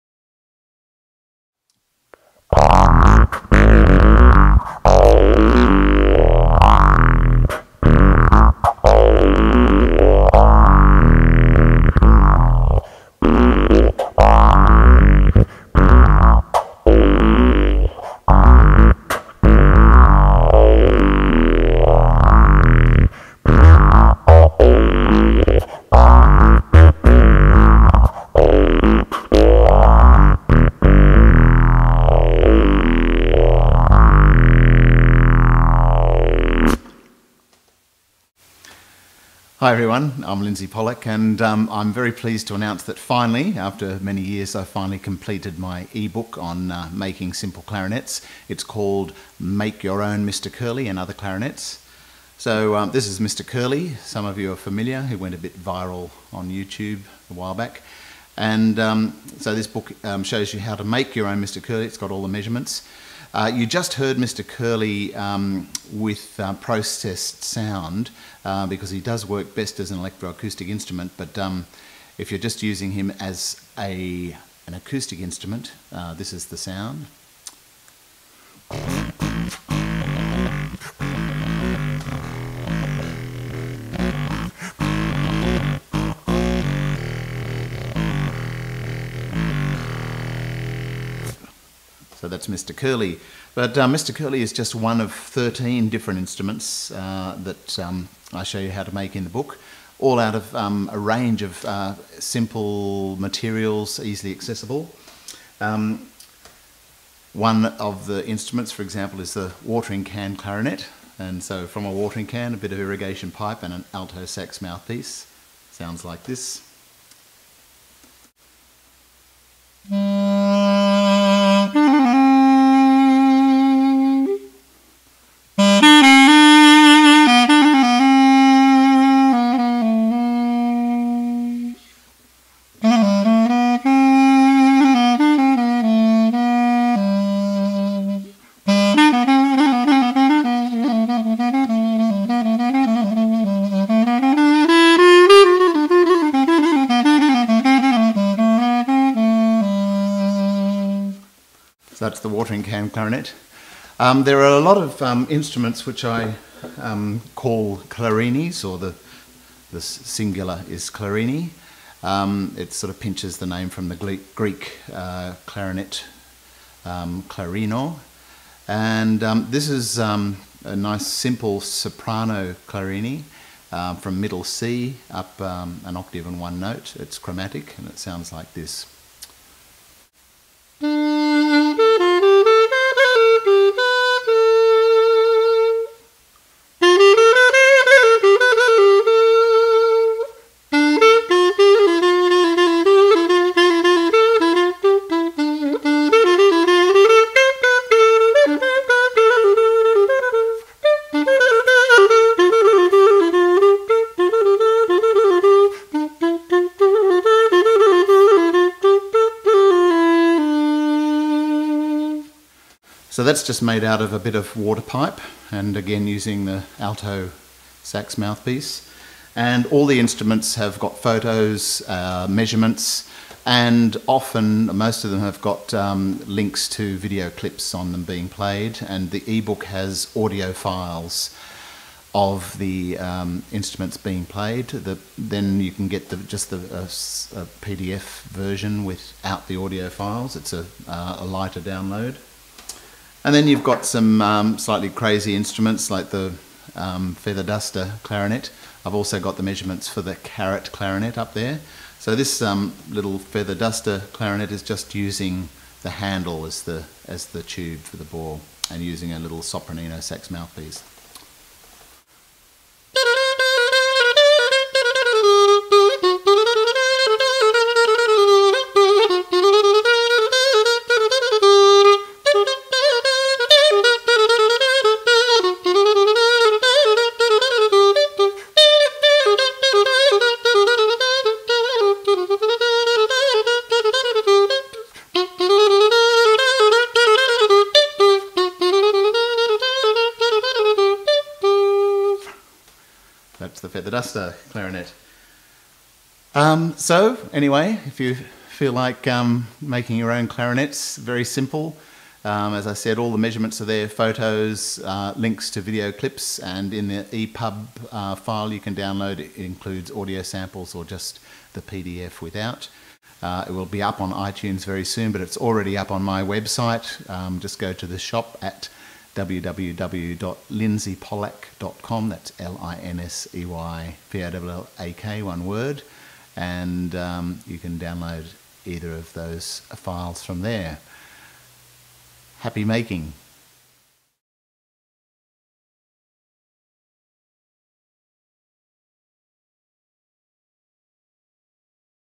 Make your own Mr Curly
in this video Linsey demonstrates some of the clarinets that the eBook shows you how to make. The eBook has measurements, photos, descriptions, video links and also audio examples of most of the instruments, such as the carrot clarinet, various clarinis, the watering can clarinet and the feather duster clarinet.